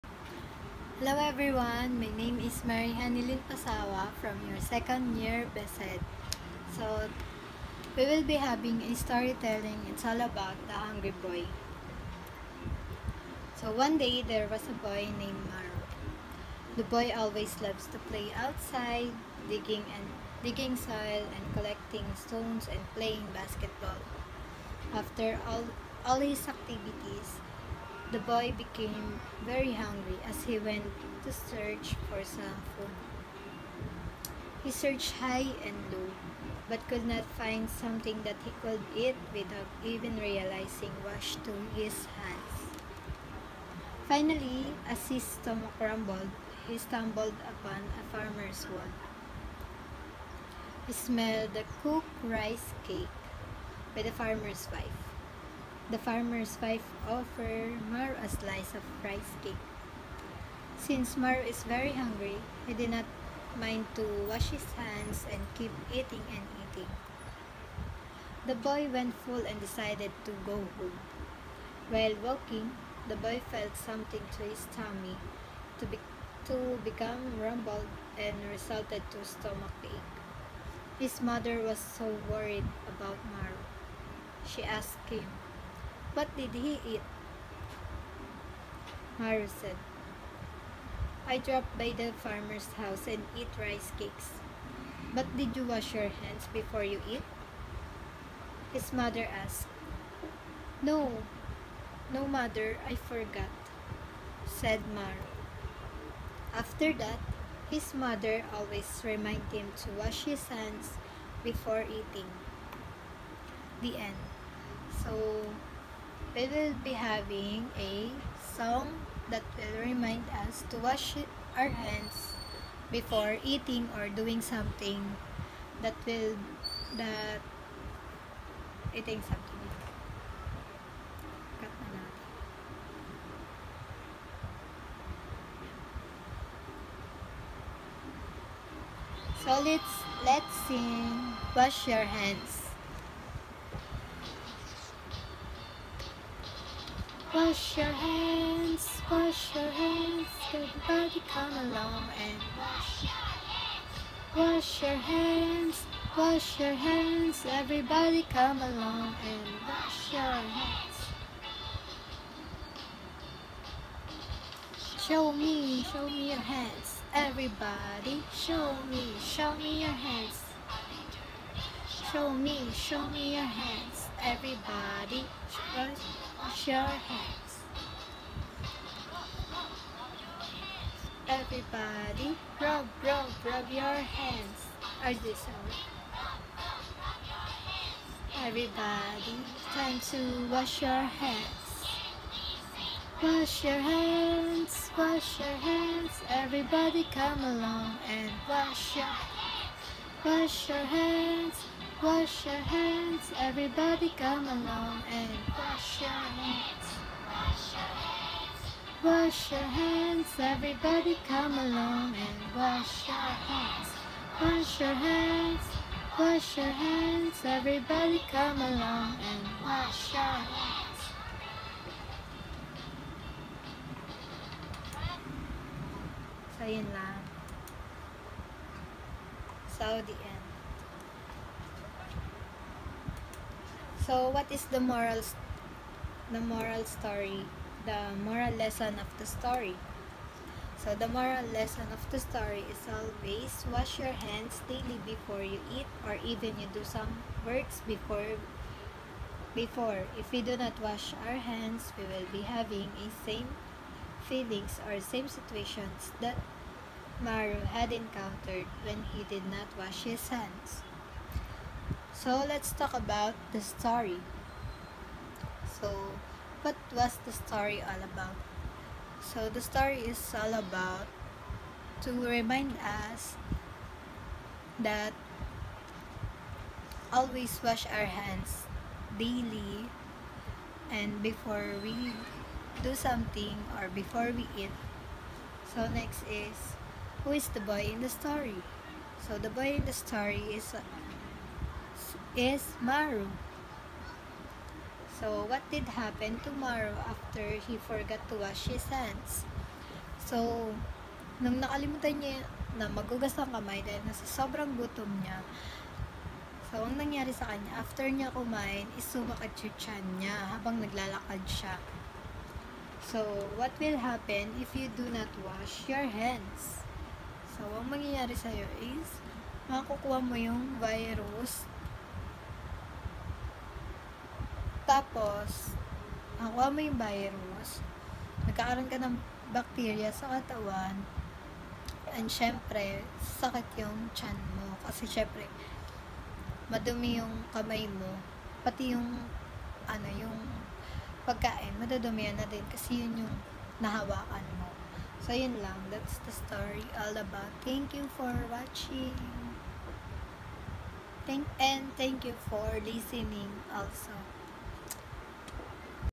Story telling presentation!❤️
Hi guys this is one of my tasks in my major subject to create storytelling that a child would learn and realize. I just want to upload here my tasks for you to watch this if you want❤️ thank you and God bless, please do comment if you enjoy this video and suggestions for my next vlog❤️